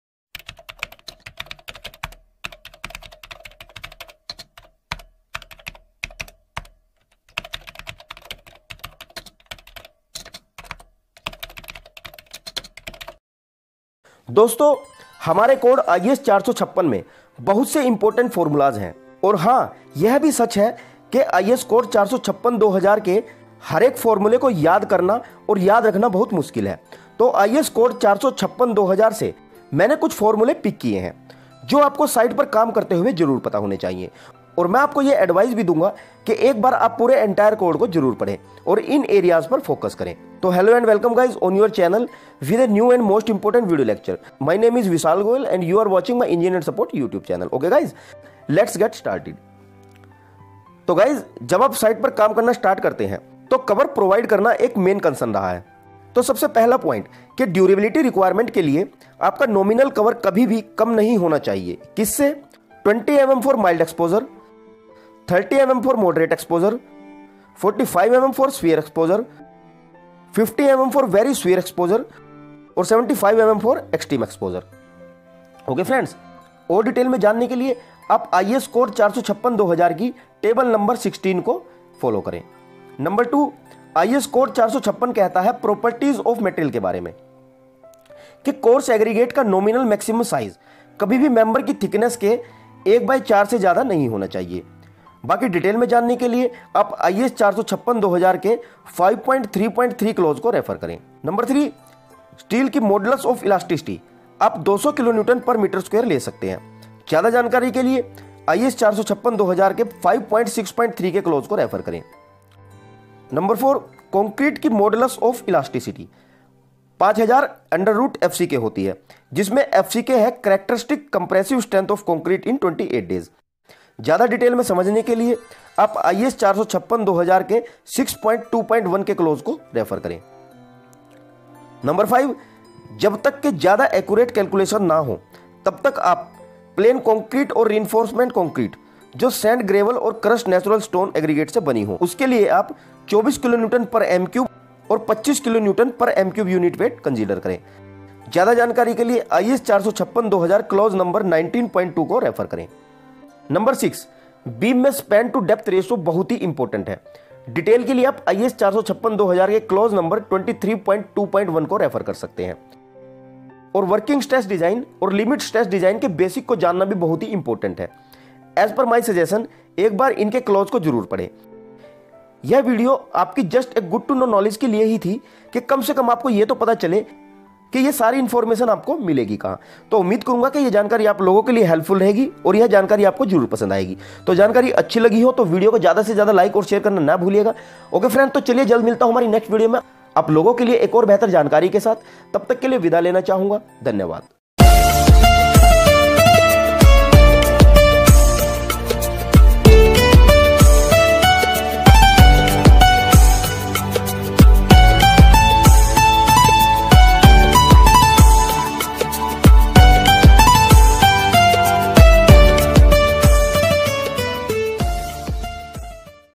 Basic Important Points For Civil Engineers & Supervisors From IS Code 456 : 2000
In this video, I will tell you, Basic Important Points For Civil Engineers & Supervisors From IS Code 456 : 2000 In Urdu/Hindi.

So Keep Watching The Video::::

Thanks